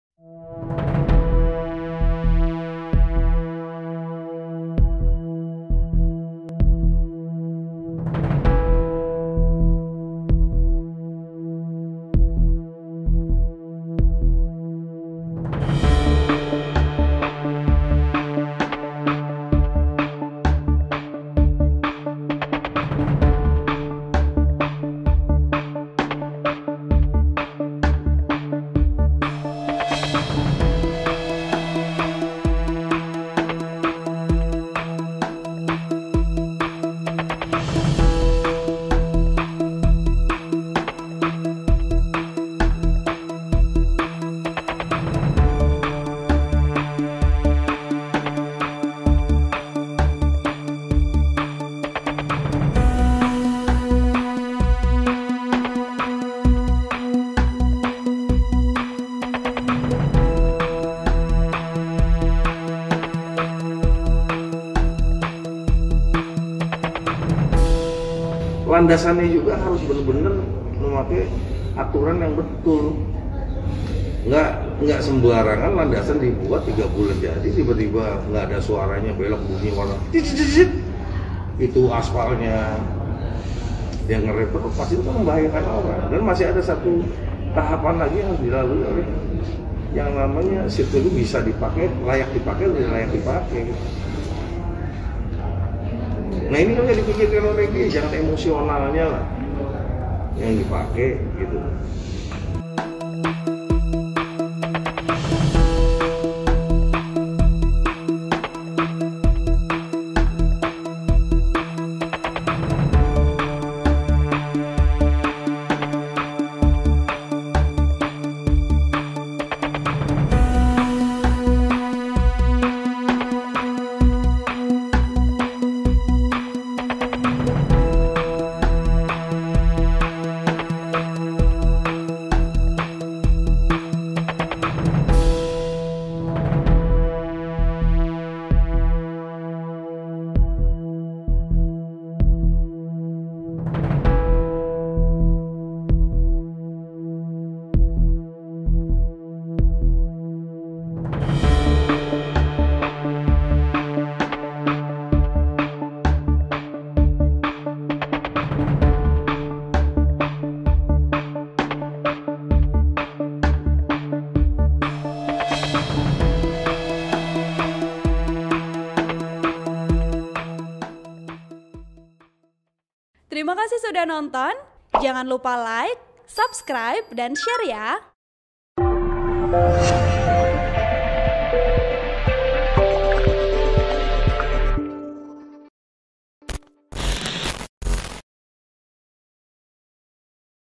Pesimis Tepat Waktu, Ketua DPRD DKI: Buat Sirkuit Formula E Bukan Seperti Buat Lintasan Tamiya
TRIBUN-VIDEO.COM - Ketua DPRD DKI Jakarta Prasetyo Edi Marsudi pesimistis penggarapan lintasan balap Formula E di Ancol tepat waktu sebelum event digelar pada 4 Juni 2022.

Menurut Prasetyo, target pembangunan sirkuit Formula E selesai dalam tiga bulan dari Pemprov DKI Jakarta adalah tak rasional.

"Buat trek balap bukan kayak buat lintasan tamiya. Rasionalnya dua tahun, bos," ujarnya saat ditemui di ruang kerjanya di Gedung DPRD DKI, Kebon Sirih, Jakarta Pusat, Rabu (26/1/2022).

Prasetyo yang juga merupakan anggota Dewan Pembina Ikatan Motor Indonesia (IMI) ini menjelaskan, Pemprov DKI Jakarta tak boleh sembarangan membuat lintasan balap.

Pasalnya, kendaraan yang akan melintas di atasnya melaju dengan kecepatan sangat tinggi.

"Kencangnya mobil Formula E itu hampir sama dengan F1. Bedanya satunya pakai mesin dan satu lagi pakai listrik," ujarnya.

Untuk itu diperlukan aspal dengan kualitas terbaik dengan konstruksi lintasan yang juga harus stabil.

Sedangkan, lokasi lintasan yang akan disulap menjadi trek balap Formula E merupakan tumpukan hasil pembuangan lumpur.

"Enggak boleh sembarangan landasan dibuat tiga bulan, nanti tiba-tiba pas lagi belok aspalnya terkelupas bagaimana," kata Prasetyo.

Hal itu disebutnya bisa membahayakan para pembalap dan bila itu terjadi maka citra Indonesia bisa jelek di mata dunia.

Oleh karena itu, Prasetyo mewanti-wanti Gubernur Anies Baswedan untuk berhati-hati dalam membuat lintasan balap Formula E.

"Jangan emosionalnya yang dipakai, ini sangat membahayakan, apalagi ini membawa nama negara yang kebetulan ada di Jakarta," tuturnya.

Wagub Ariza Optimis Trek Formula E Selesai Tepat Waktu

Wakil Gubernur DKI Jakarta Ahmad Riza Patria tetap optimis Formula E bisa terselenggara dengan baik meski proses tender pembuatan sirkuitnya gagal.

"Tentu harus optimis ya, karena optimis itu menghadirkan energi positif yang baik," ucapnya di Balai Kota, Rabu (26/1/2022).

Ariza pun mengibaratkan penyelenggaraan Formula E dengan upaya pemerintah membangun bangsa.

Menurutnya, apapun tantangan yang menghadang pemerintah akan menghadapinya dengan rasa optimis.

"Dalam membangun bangsa, kita harus selalu optimis ya. Membangun bangsa, membangun kota, membangun daerah, membangun desa, kita harus optimis," ujarnya.

"Apapun tantangan yang kita hadapi, betapapun beratnya, kita harus kerja sama bersinergi, berkolaborasi, dan berjuang," sambungnya.

Orang nomor dua di DKI ini pun mempercayakan penyelenggaraan Formula E ini kepada BUMD PT Jakarta Propertindo (Jakpro).

Meski proses tender sempat gagal, Ariza optimis, lintasan balap mobil bertenaga listrik akan rampung sesuai dengan target yang sudah ditetapkan.

"Kita serahkan saja kepada panitia yang lebih memahami secara teknis. Tugas kita mari kita berikan support, dukungan, dan doa," tuturnya.

BUMD PT Jakarta Propertindo (Jakpro) akui dana untuk pembangunan Formula E dari sponsor belum masuk.

Hal ini diungkap oleh Direktur Pengembangan Bisnis PT Jakpro sekaligus Managing Director Formula E, Gunung Kartiko.

"Ya jadi total yang dibutuhkan dengan yang kemarin sampai dengan sekarang itu sekitar Rp 150 miliar (dana pembangunan trek secara total). Sebagian itu sudah dibeli sejak tahun 2019, sekitar Rp 70 miliar. Nah ini akan kita tutup rencananya ini dari sponsorship dan partnership," jelasnya di ruang rapat Komisi B DPRD DKI, Senin (24/1/2022).

Pernyataan itu pun terus dicecar oleh Komisi B DPRD. Di mana anggota Komisi B DPRD Fraksi PDIP, Gilbert Simanjuntak menanyakan kelanjutan sponsorship yang disebutkan.

Pasalnya, dana untuk Formula E memang digadang-gadang dari sponsor dan tak memakai APBD DKI 2022.

Hal ini lantaran belum ada hitam diatas putih mengenai perjanjian dengan pihak sponsor.

"Jadi sponsorship belum secara resmi kita open, tapi secara verbal secara pendekatan networking yang berminat walaupun belum bisa kita declare (umumkan) disini, karena belum hitam di atas putih," ungkapnya.

Talangi Pembuatan Trek Formula E

Artikel ini telah tayang di TribunJakarta.com dengan judul Pesimis Tepat Waktu, Ketua DPRD DKI: Buat Sirkuit Formula E Bukan Seperti Buat Lintasan Tamiya,
Penulis: Dionisius Arya Bima Suci | Editor: Acos aka Abdul Qodir